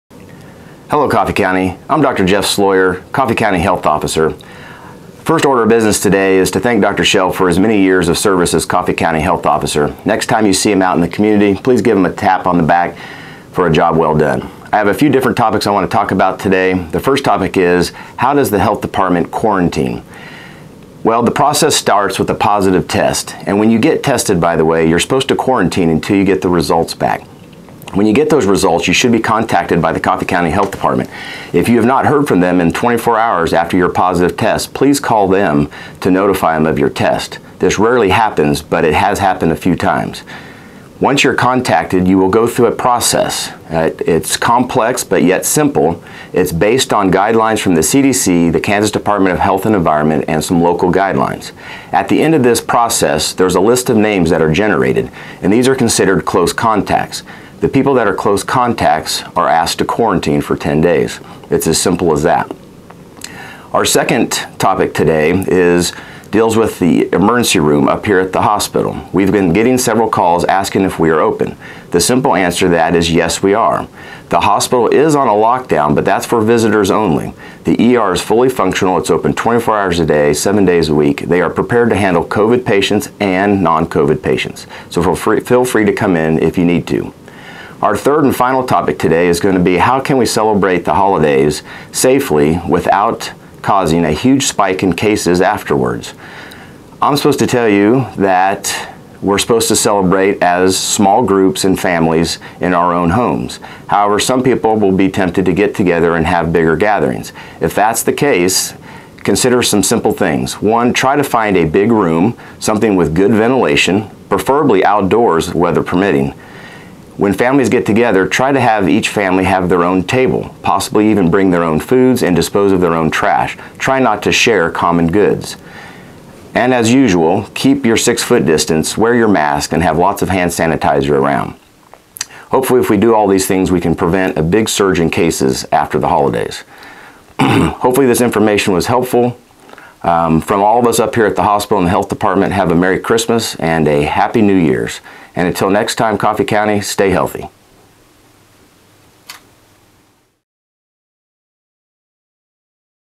Tips from Dr. Jeff Sloyer
Coffey County Health Officer Jeff Sloyer, M.D. offers COVID information and advice for staying safe and healthy.